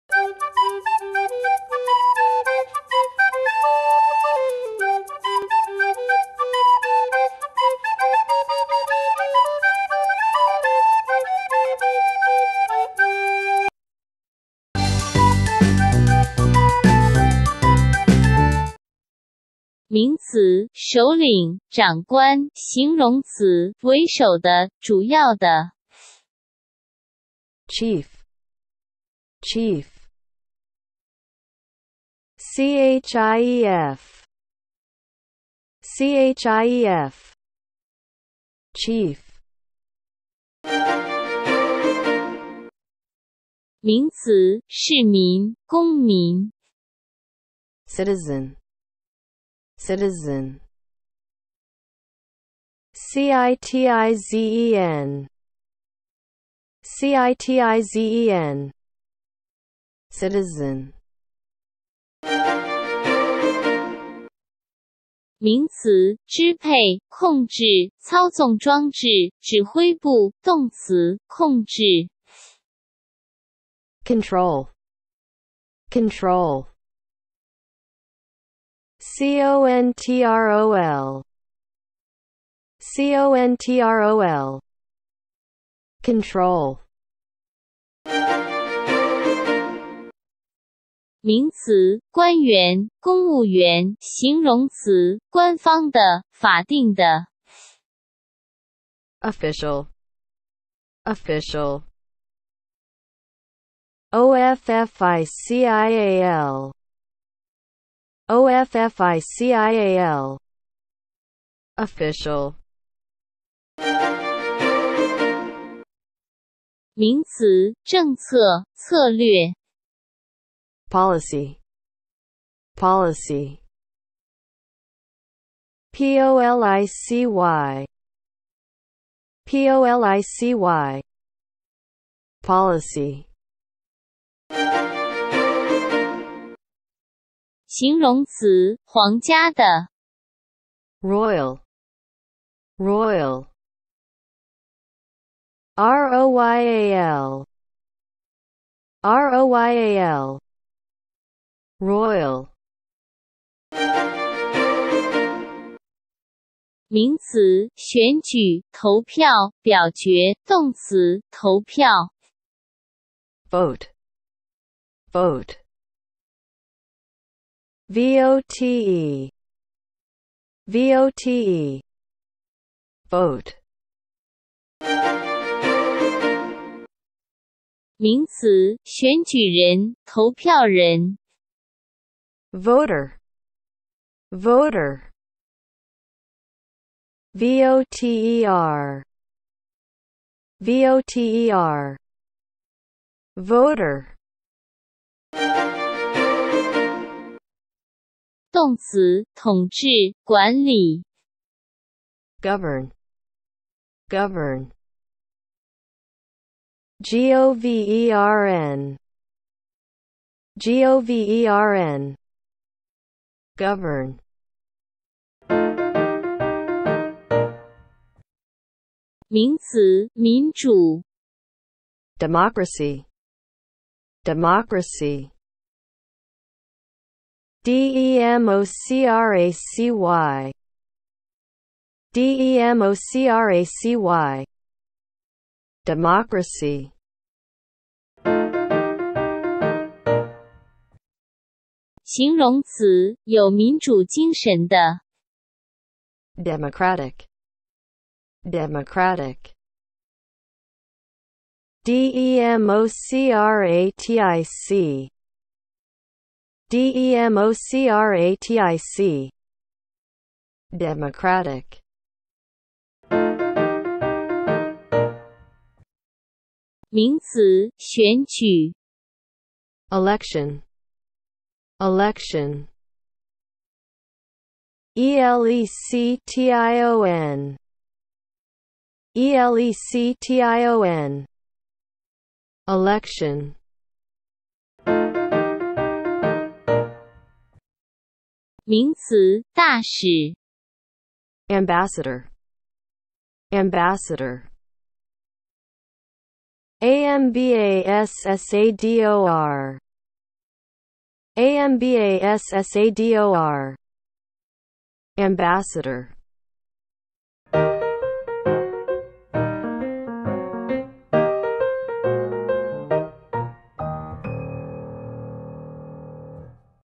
經典單字(高中7000字)-第169單元(中前英後)
【chief】
直接播放：0:14

【citizen】
直接播放：0:40

【control】
直接播放：1:02

【official】
直接播放：1:27

【policy】
直接播放：1:53

【royal】
直接播放：2:14

【vote】
直接播放：2:33

【voter】
直接播放：2:54

【govern】
直接播放：3:15

【democracy】
直接播放：3:35

【democratic】
直接播放：3:58

【election】
直接播放：4:22

【ambassador】
直接播放：4:44